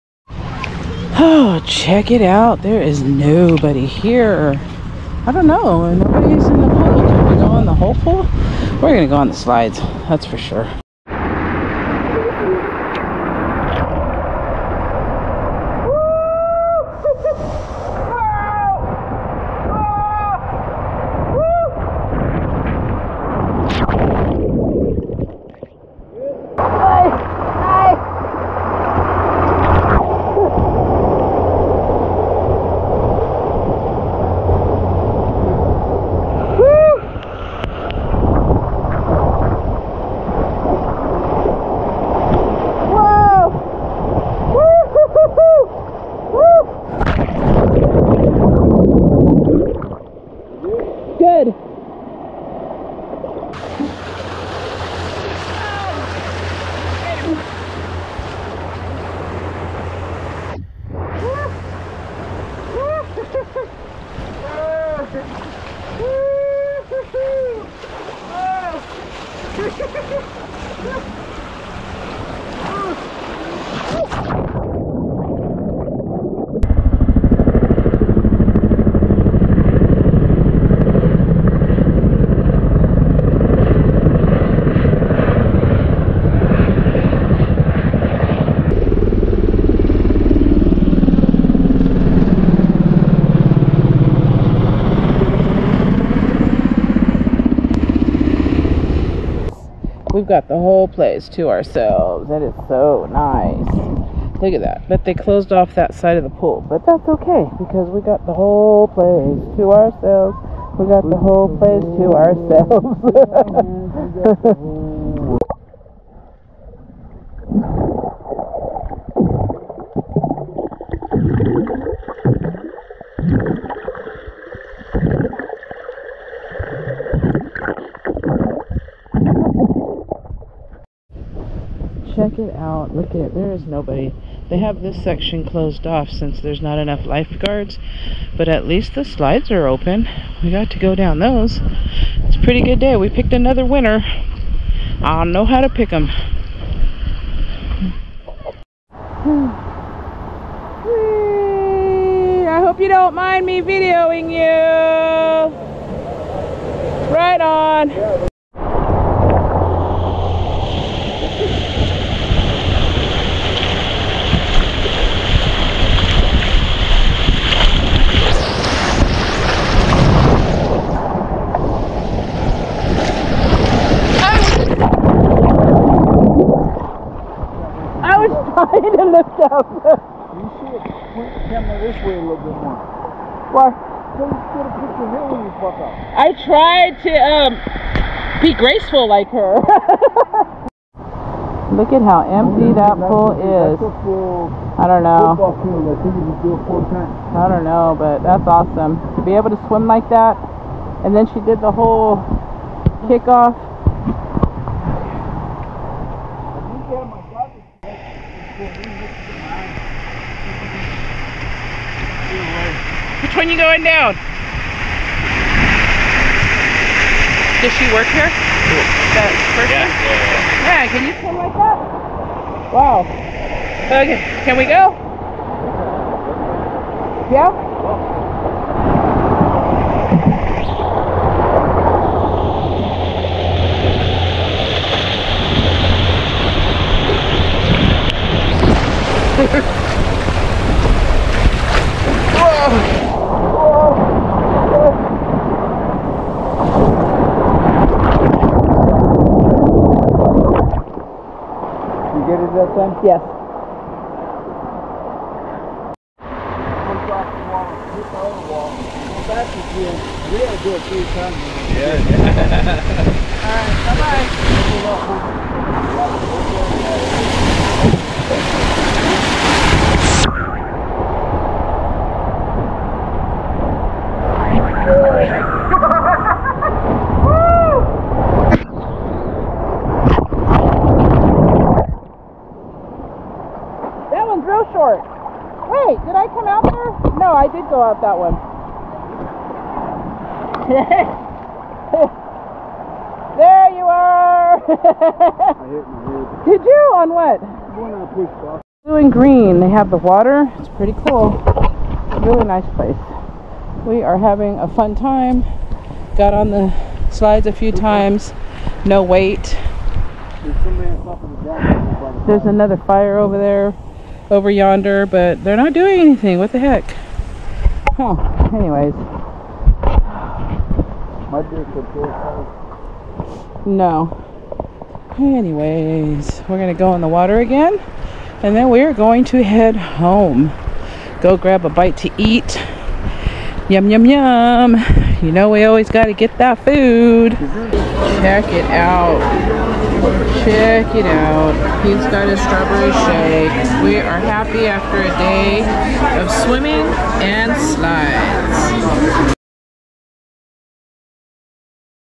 SLIDES at HANSEN DAM!!!
Wanted to go ONE MORE TIME before summer was officially over. Picked a GREAT day. Had the place all to ourselves. It was WONDERFUL.